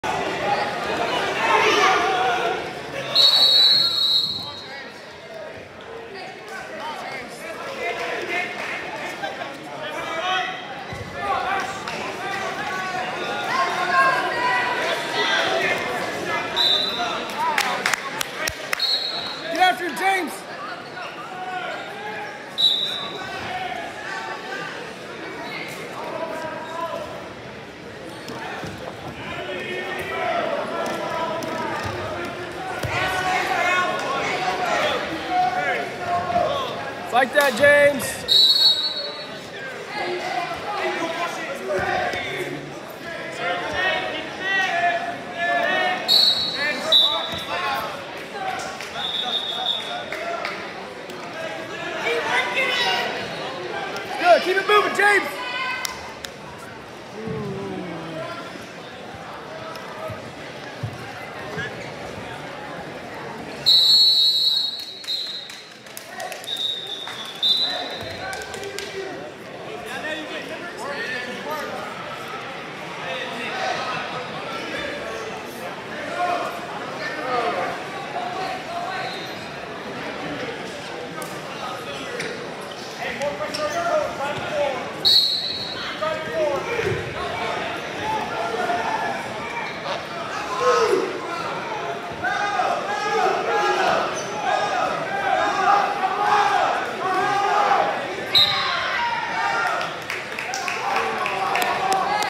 190 Adams IV, James v Marjan, Z. (Athens Drive) 02-10-24 L PIN 1:21
NCHSAA 4A East Regional - Varsity Tournament 2/9/24, 5:26 PM